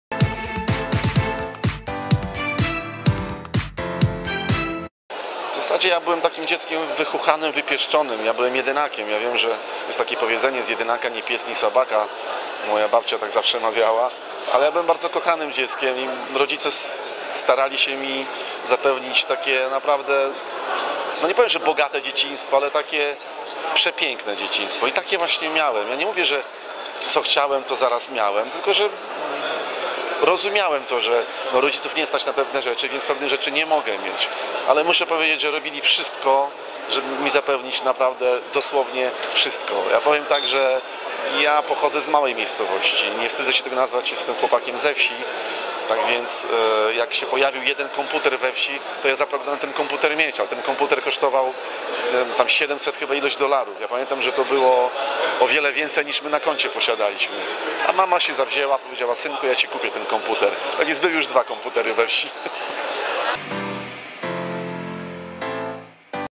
Jakim dzieckiem był Marcin Miller z zespołu Boys? HD
Jesteście ciekawi, jakie dzieciństwo miał Marcin Miller? Jako mały chłopiec żył w biedzie czy w dostatku? Zobaczcie nasz wywiad.

- Najlepsze wywiady, plotki z życia gwiazd disco polo, relacje z koncertów.

Chcesz zareklamować się na naszym portalu?